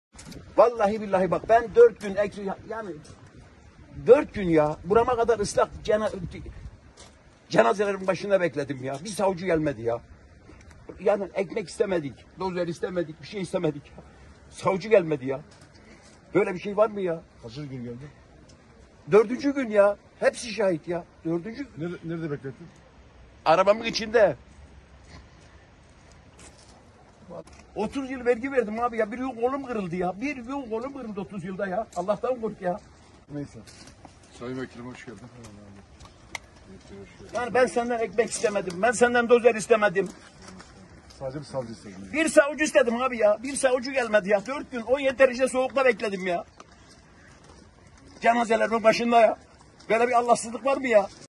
Ağbaba’nın Dinlediği Depremzede: Ben Dört Gün, Cenazelerin Başında Bekledim, Bir Savcı Gelmedi
CHP Genel Başkan Yardımcısı Veli Ağbaba’nın, Malatya Erkenek Mahallesi’nde dinlediği depremzede; “Ben dört gün, cenazelerin başında bekledim, bir savcı gelmedi. Ekmek istemedik, dozer istemedik, su istemedik… Savcı gelmedi. Böyle bir şey var mı?” dedi.